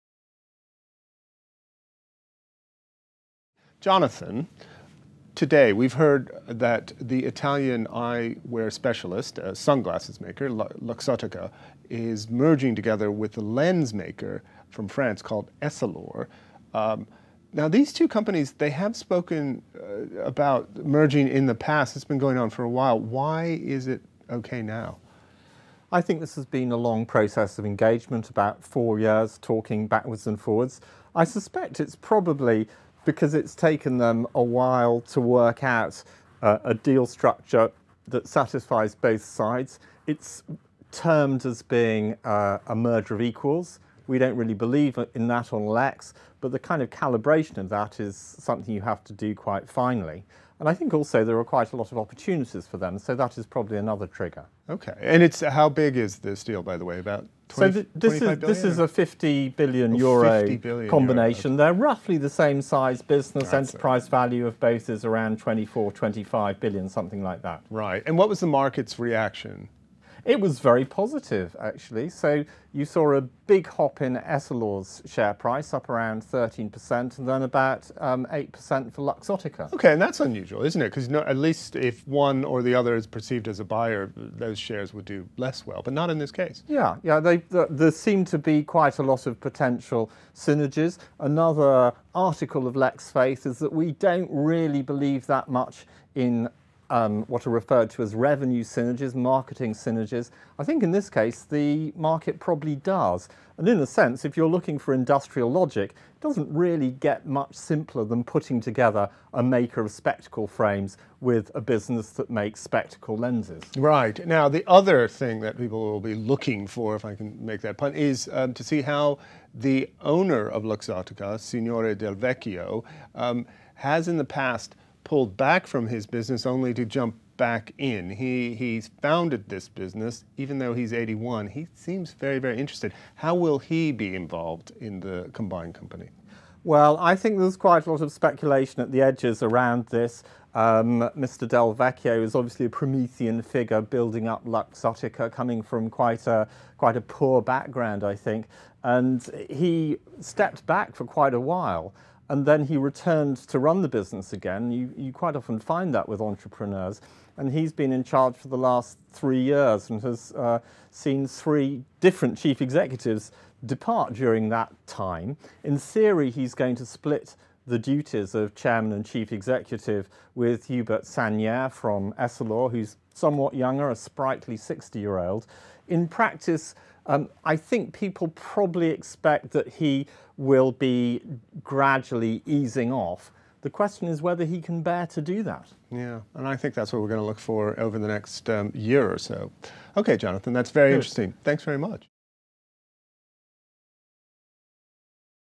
Luxottica and Essilor agree €50bn merger | Lex
Lex’s Jonathan's Guthrie and Alan Livsey discuss the €50bn merger between Luxottica, the leading consumer eyewear group and owner of Ray-Ban, Oakley and Sunglass Hut, and Essilor, the leading manufacturer of lenses